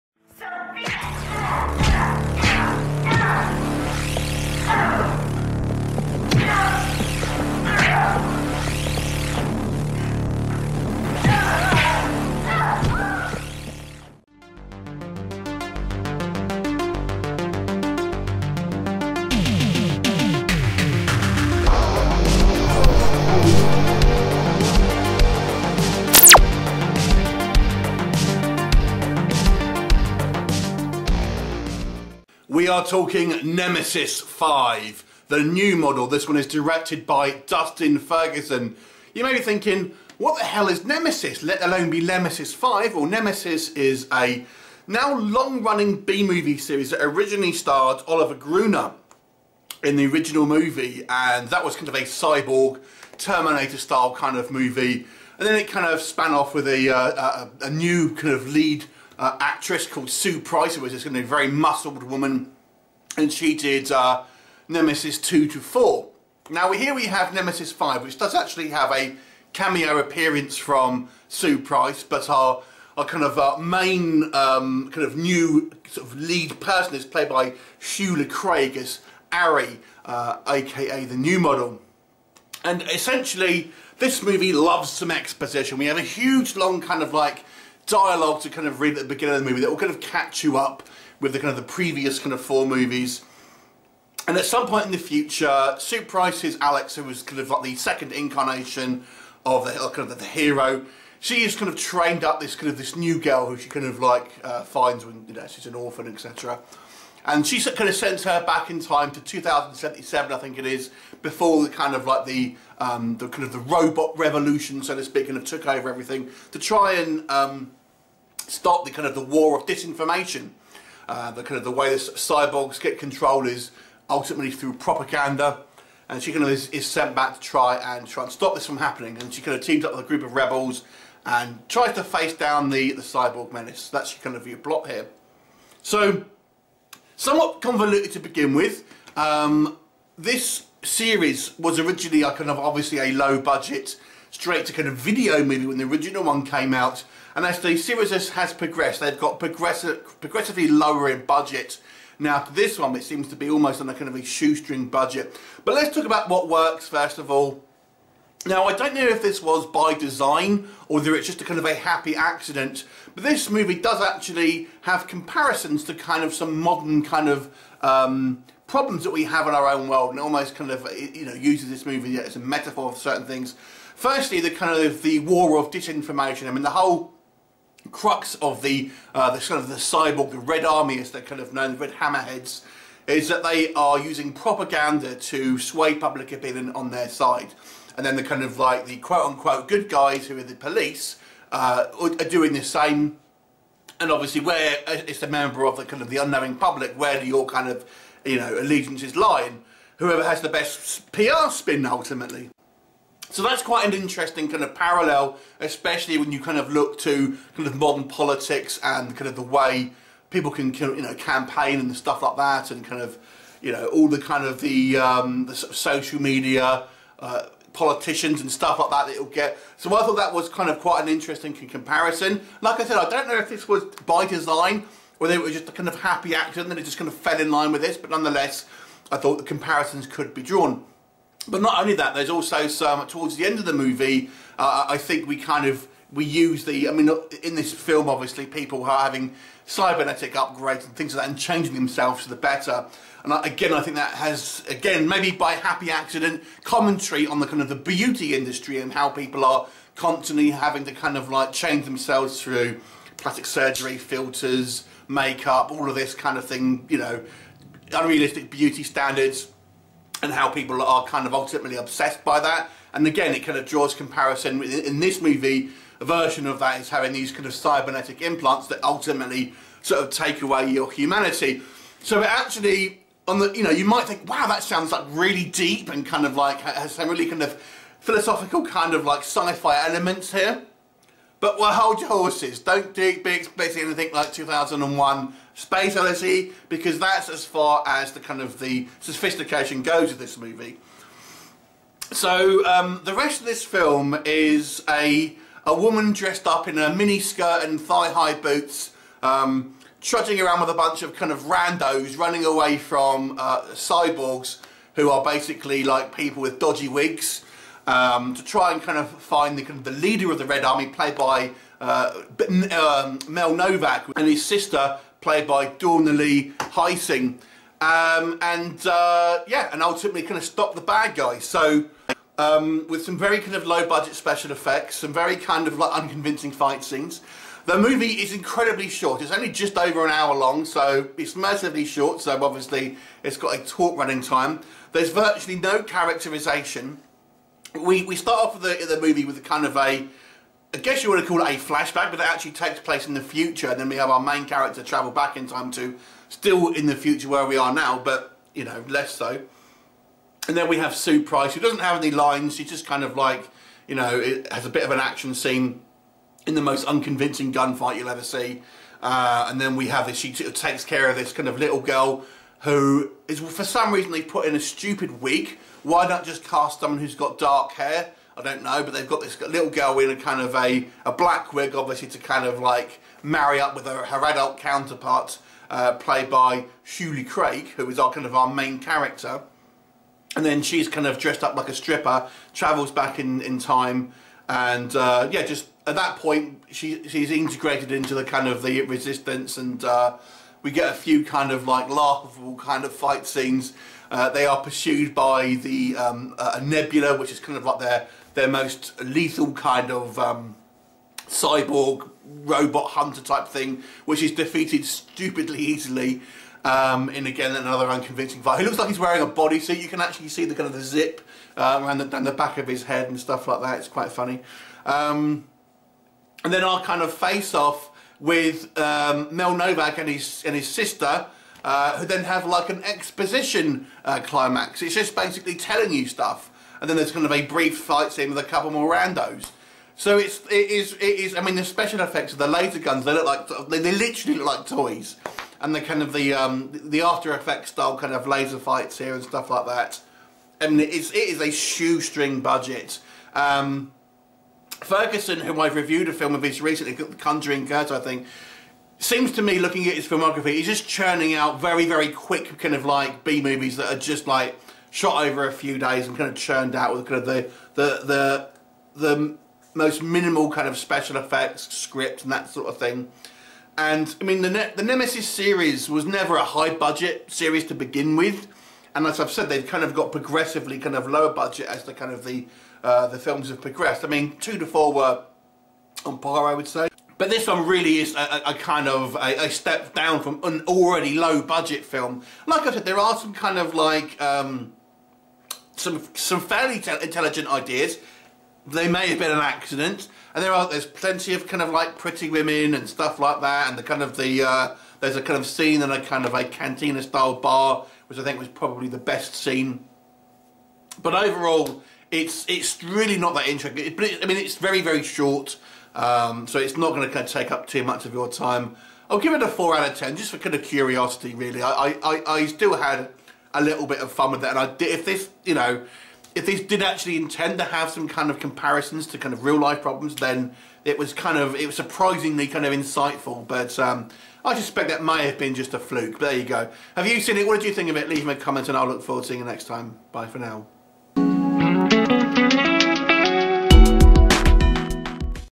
NEMESIS 5 : THE NEW MODEL ( 2017 Schuylar Craig ) Cyborg Sci-Fi Movie Review
It is the year 2077 and terrorist organization known as The Red Army Hammerheads now dominate the Earth. The fate of humanity now rests in the hands of The New Model, Ari Frost. Trained under the wing of Alex Rain, she must form an army of Cyborg Humans to take down the regime and regain control. After all, 86.5% is still considered human.
Released 21 years after the previous installment